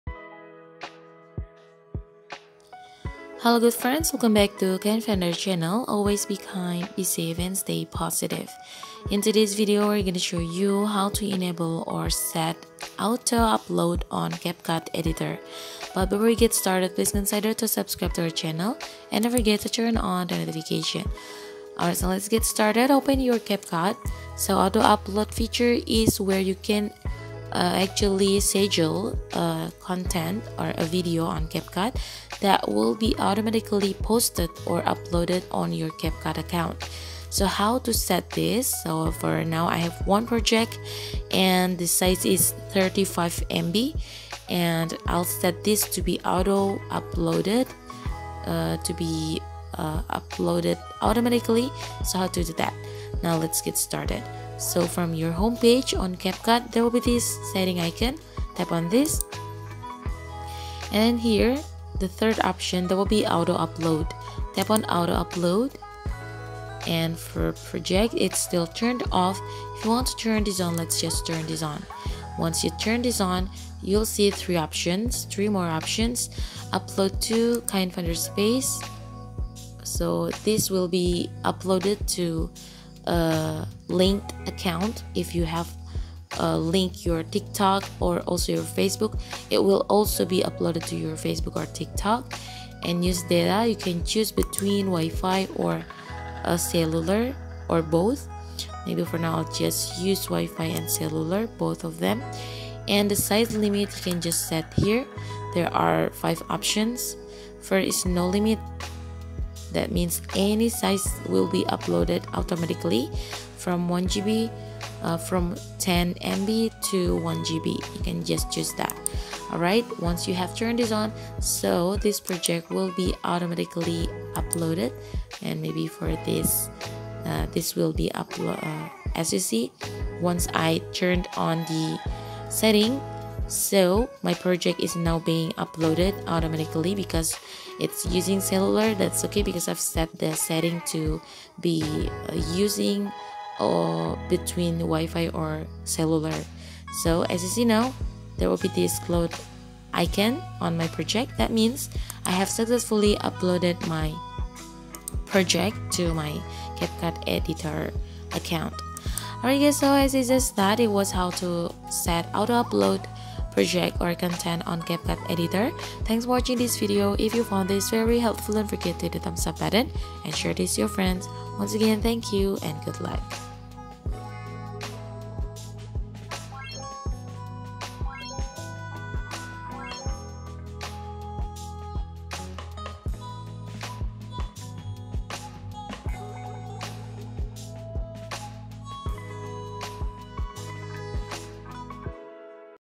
Auto Upload Video Using CapCut Editor
How to Auto Upload Video Using CapCut Editor

This video shows you how to Auto Upload Video Using CapCut Editor

If you want to know how to Auto Upload Video Using CapCut Editor please follow the steps in this video carefully.

Sincerely
Kind Finder